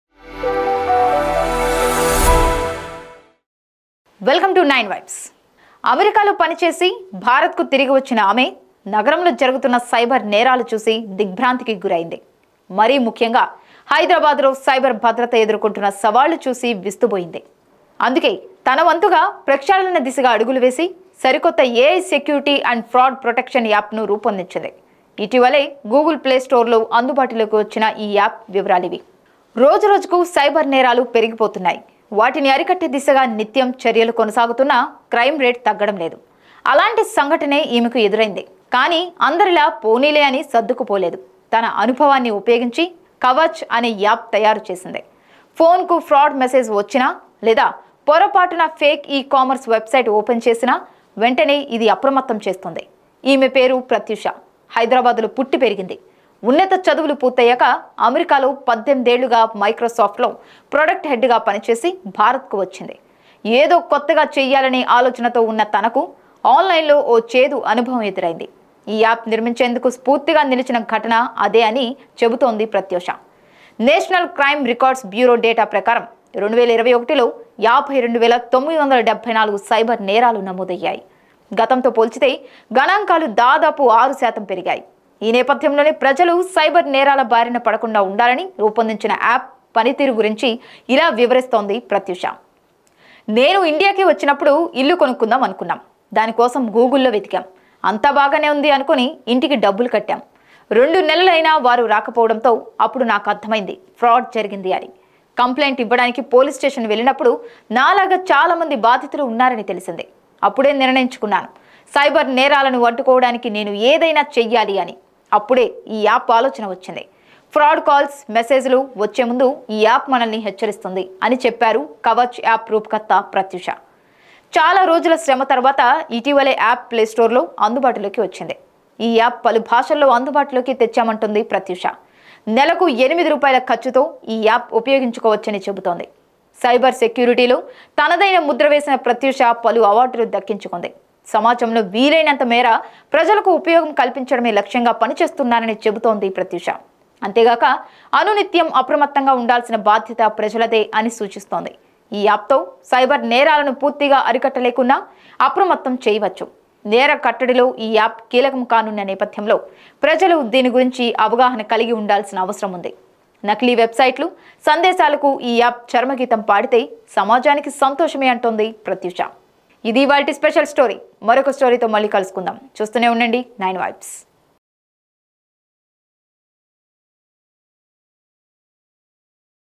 Kavach Application: How to Protect Your Data and Devices from Cyber Threats with AI
in this video, you will learn about Kavach Application, a multi-factor authentication app that helps you secure your access to various software solutions across your IT landscape. Kavach Application is developed by the National Informatics Center and works with Kavach Authentication Server in the backend. You will also discover how Kavach Application uses AI to detect and prevent fraud and threats to your data and devices